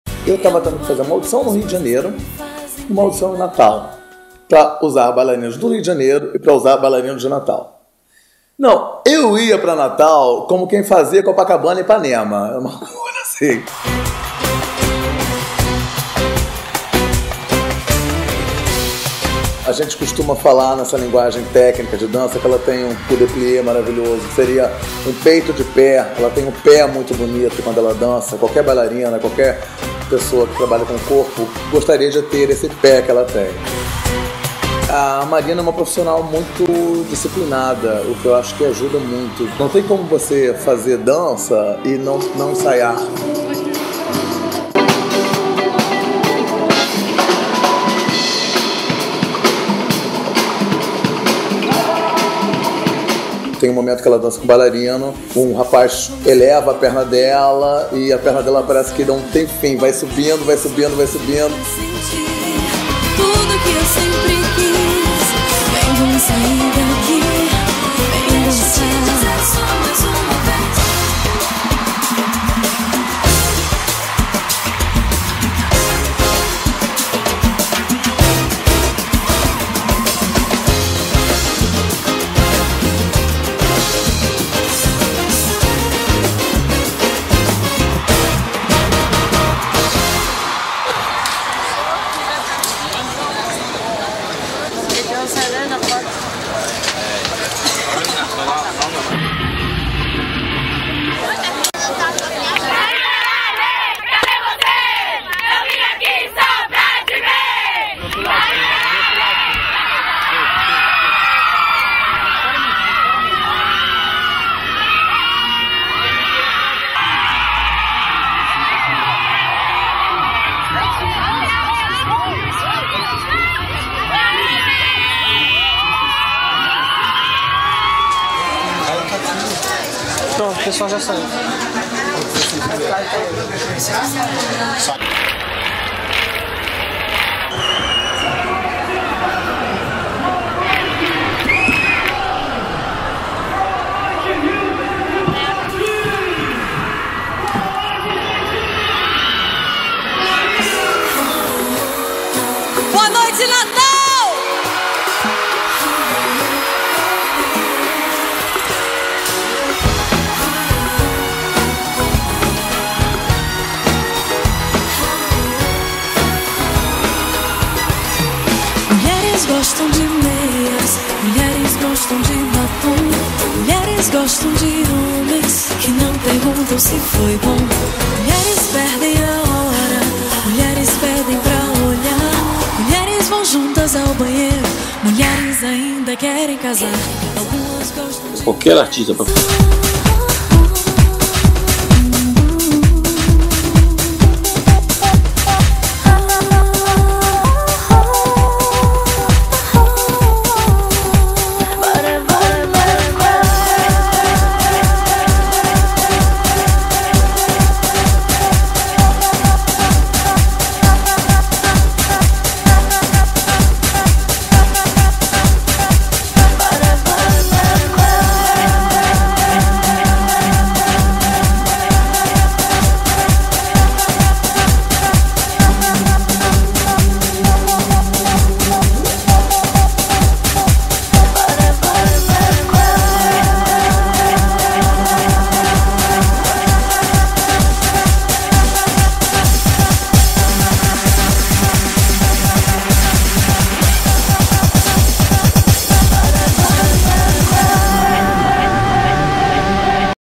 Marina Elali Concert Live DVD Documentary part 2
This video is from Marina Elali Concert Live DVD,in 15th December,2007 ,in João Machado Stadium,Natal-RN. Director:Roberto Carminati. Choreogrepher:Caio Nunes. Dancers:Michael Ximenes,Álvaro Paraguai,Alexandre Lima,Alex Rocha,Gil Soares,Henrique Paranhos,João Alexandre,Leandro Azevedo,Leonardo Vieira,Monir Cavalcante,Rafael dos Santos,Rodrigo Silbat,Ronaldo Damas and Vitor Hugo.